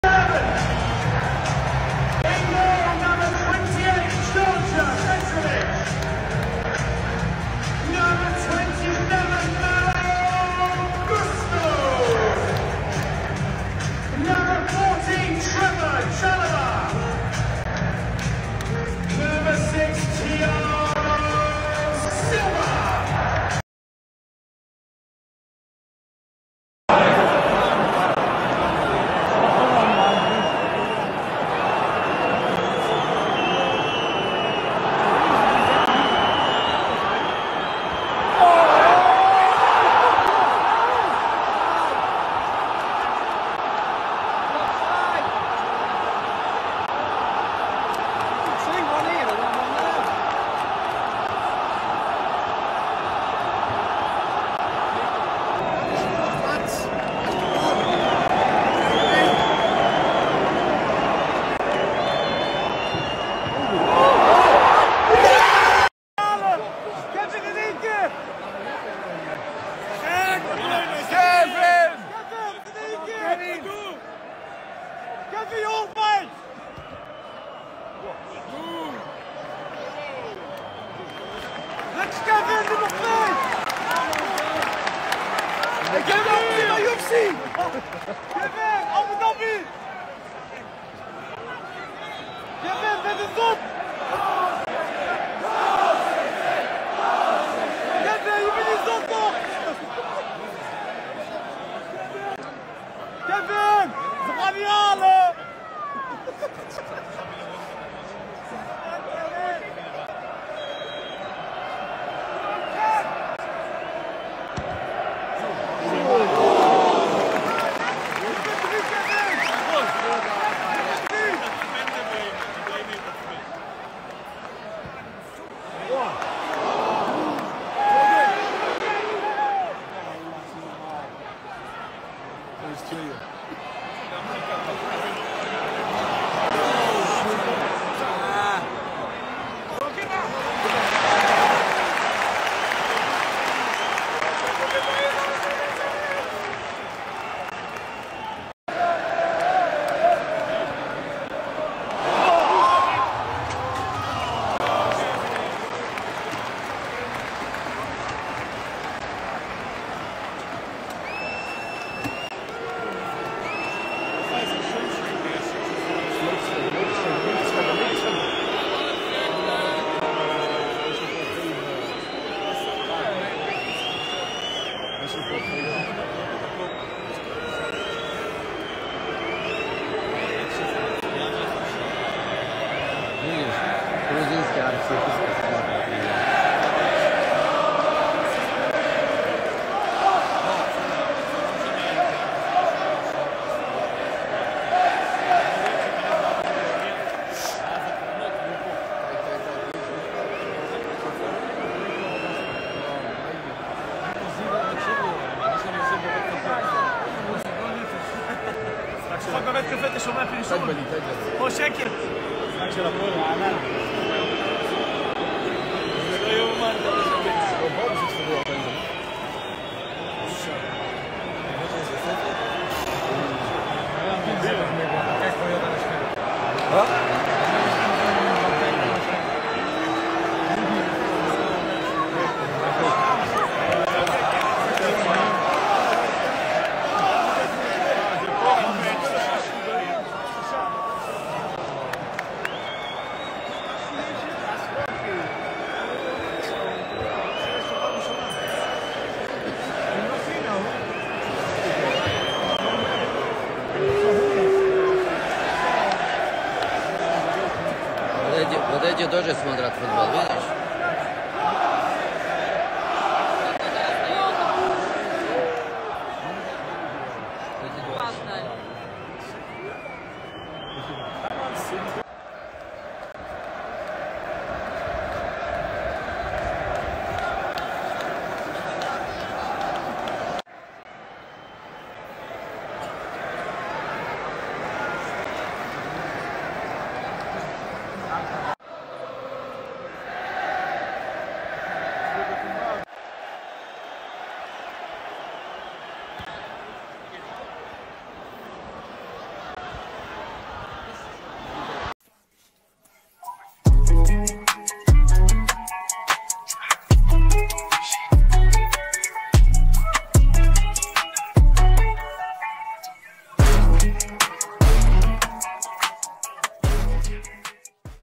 Manchester City vs Real Madrid 1-1 (3-4) - All Goals and PENALTY Highlights - 2024 🔥 DE BRUYNE
11 000 Likes for Man City vs Real Madrid 2024?
This is my Vlog Video From Etihad Stadium, Manchester, England Ma City vs Real Madrid penalty shootout goals and highlights 2024 Enjoy it!